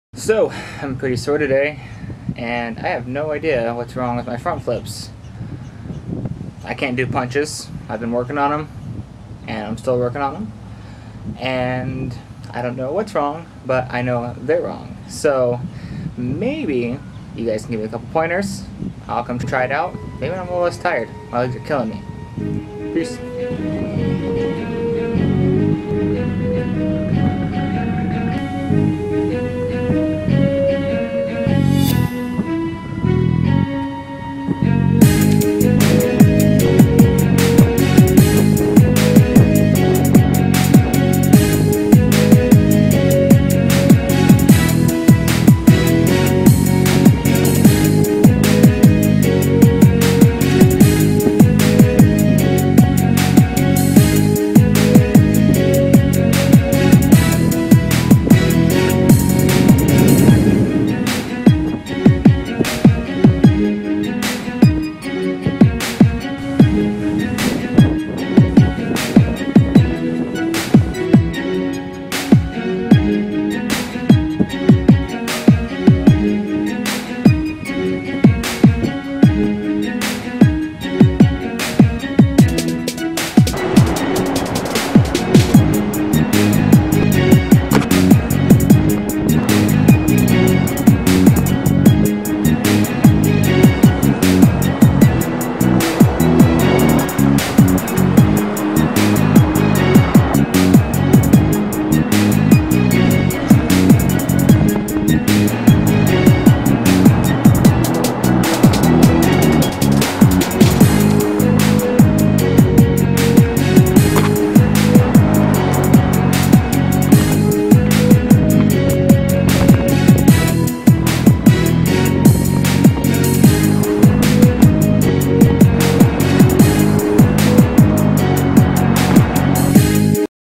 Training Day 5 Front Flips
I was really sore and working on some fronts this is how it turned out